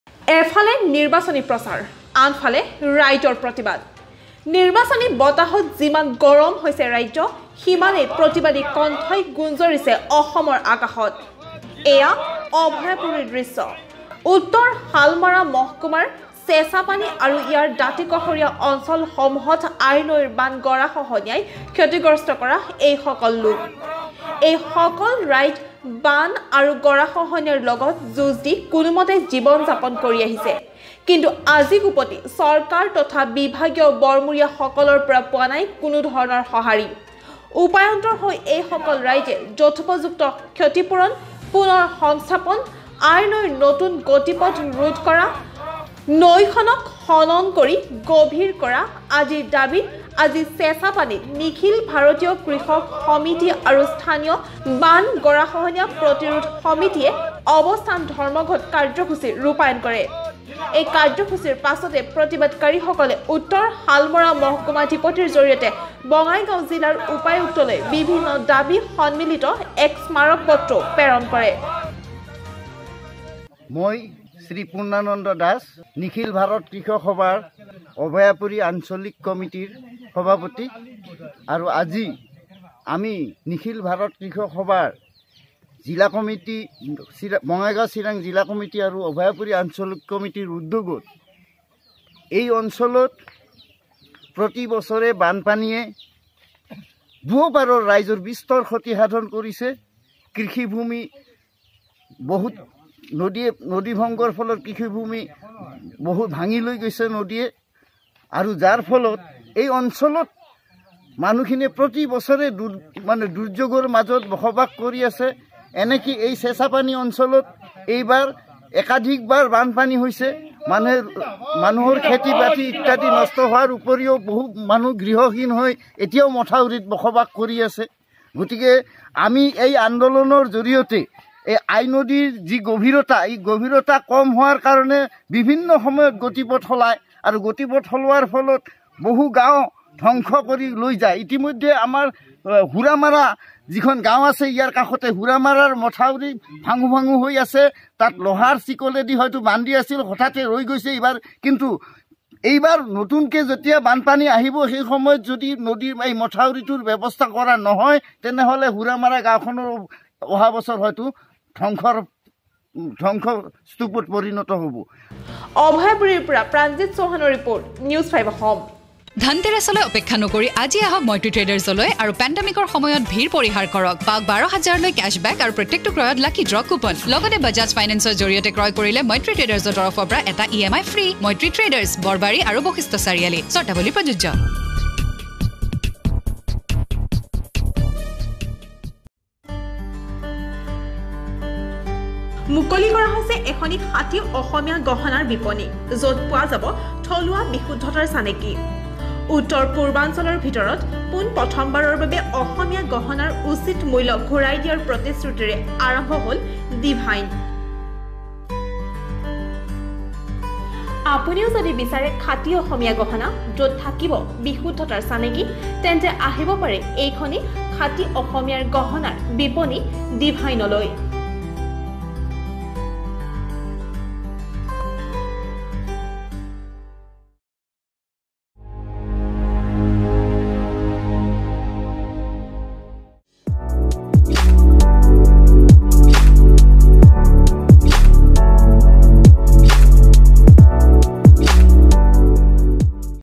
এফালে নিৰ্বাচনী প্ৰচাৰ, আনফালে ৰাইজৰ প্ৰতিবাদ।
নিৰ্বাচনী বতাহত যিমান গৰম হৈ আছে ৰাজ্য, সিমানেই প্ৰতিবাদী কণ্ঠই গুঞ্জৰিছে অসমৰ আকাশত।
এয়া অভয়াপুৰীৰ দৃশ্য।
উত্তৰ শালমাৰা মহকুমাৰ চেচাপানী আৰু ইয়াৰ দাতি কাষৰীয়া অঞ্চল সমূহত আইনৈৰ বান খহনীয়াই ক্ষতি কৰা এই সকল ৰাইজ।
এই সকল ৰাইজে বান আৰু গৰাখনিয়াৰ লগত যুজঁ দি কোনোমতে জীৱন যাপন কৰি আছে। কিন্তু আজি কুপতি চৰকাৰ তথা বিভাগীয় বৰমুৰীয়া সকলৰ পৰা পোৱা নাই কোনো সঁহাৰি।